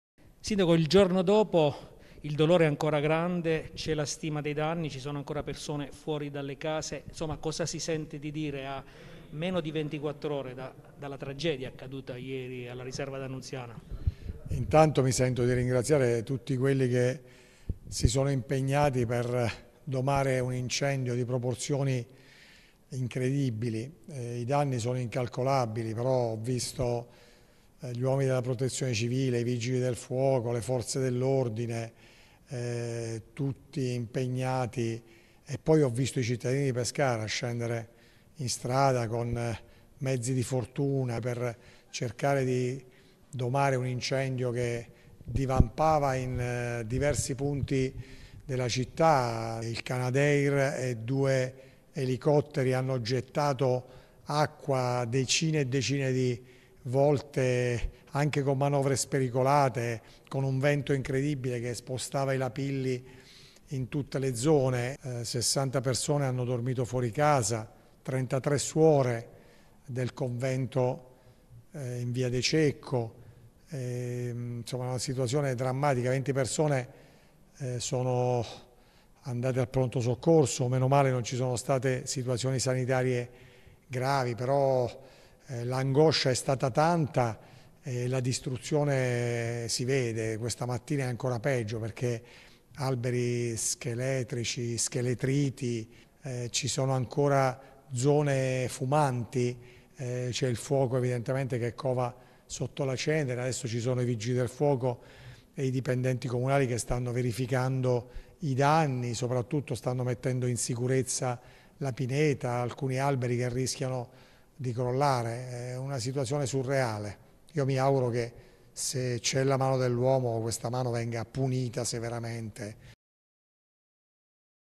Incendio Pescara. Masci, danni incalcolabili
Il sindaco Pescara sta seguendo minuto dopo minuto l’evolversi della situazione dopo l’incendio che ieri ha investito la parte sud della città. Ci sono ancora numerose persone fuori di casa